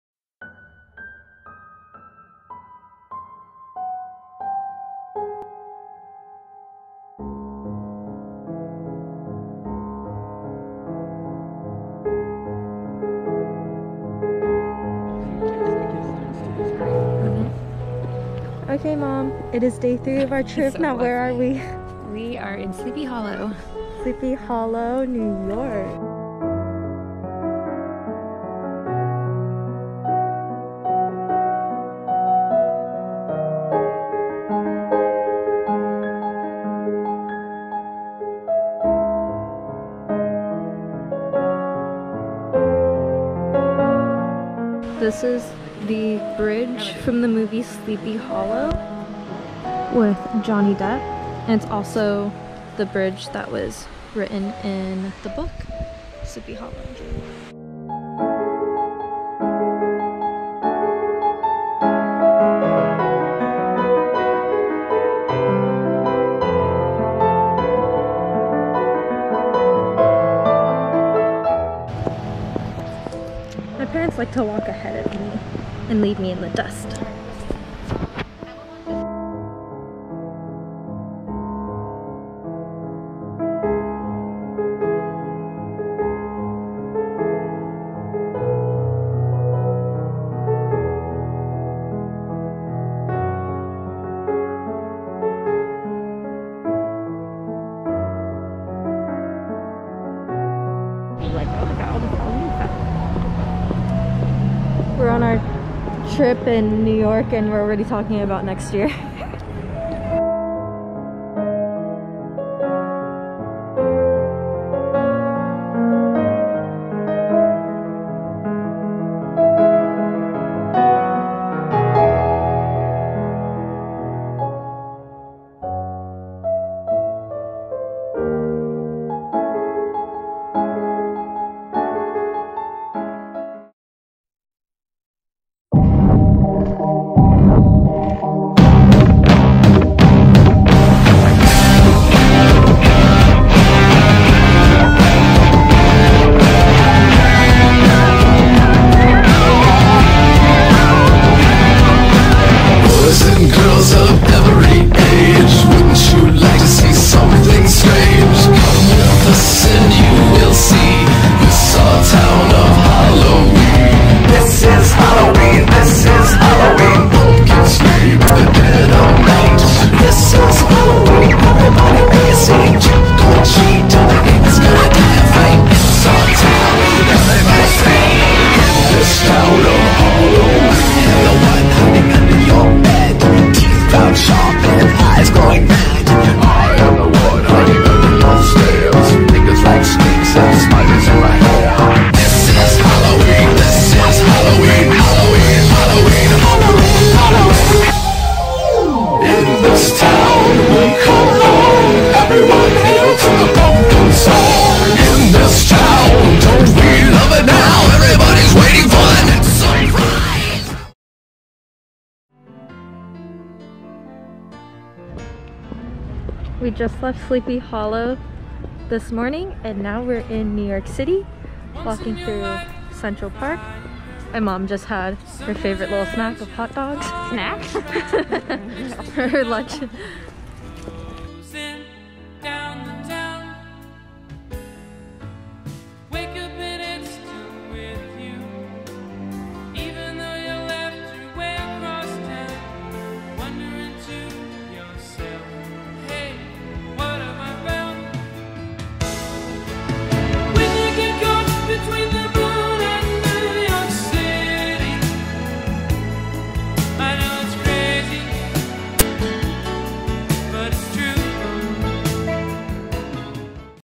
SLEEPY HOLLOW, NY VLOG
Part two of our East Coast Halloween trip.. Visiting Sleepy Hollow, NY.
The village that inspired the Legend of Sleepy Hollow story and inspiration for the movie.